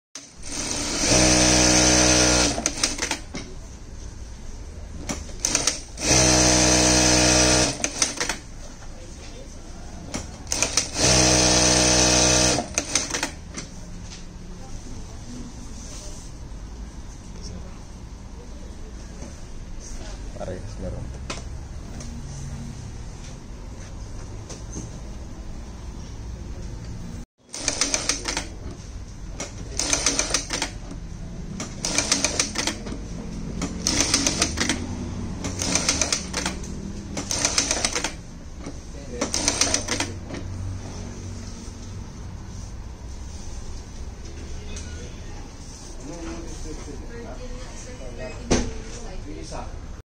Bruce R2 - HIGH SPEED COMPUTERIZED LOCKSTITCH MACHINE Demo
- High Speed Computerized Lockstitch Machine *Direct Drive Motor*
- German Technology, stable and durable
- Needle position, easy to adjust

*Auto thread trimming*
*Auto Reverse*
*Electronic thread tension*

Complete Package (Table, Built-in LED light, Thread Stand Setup)